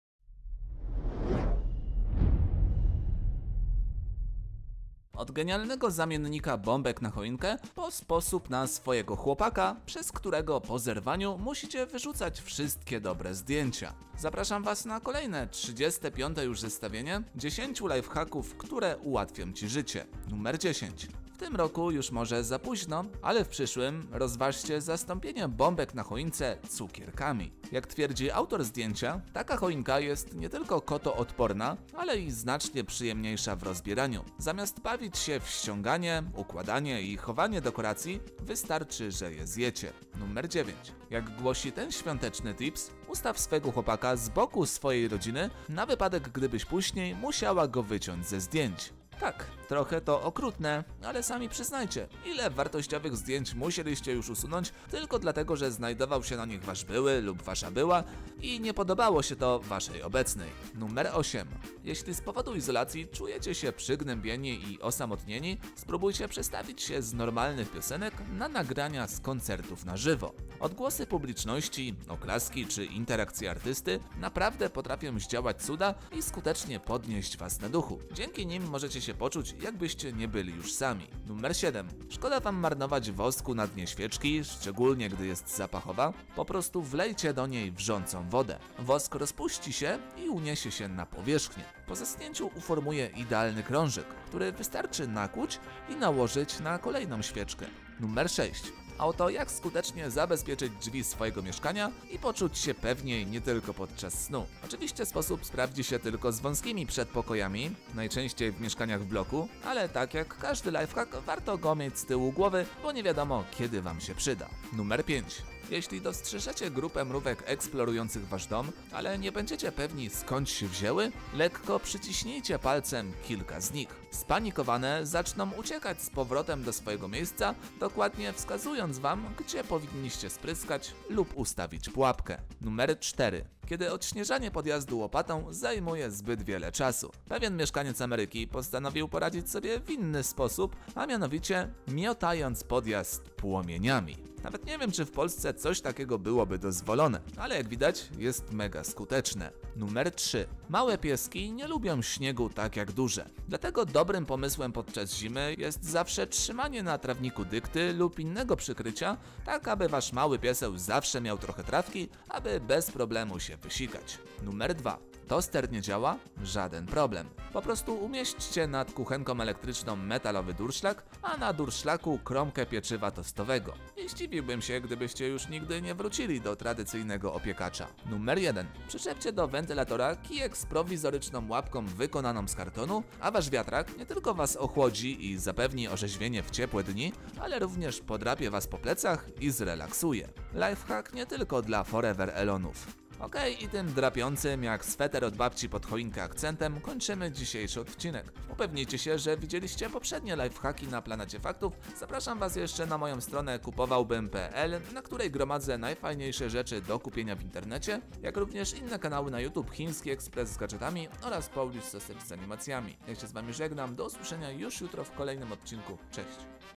10 lifehacków, które ułatwią ci życie, cz. 35
Od genialnego zamiennika bombek na choinkę, po sposobu na swojego chłopaka, przez którego po zerwaniu musicie wyrzucać wszystkie dobre zdjęcia. Zapraszam was na kolejne, 35. już zestawienie 10 lifehacków, które ułatwią ci życie.

Muzyka w tle:
(biblioteka YouTube)

Pozdrawiam,
Jacek Makarewicz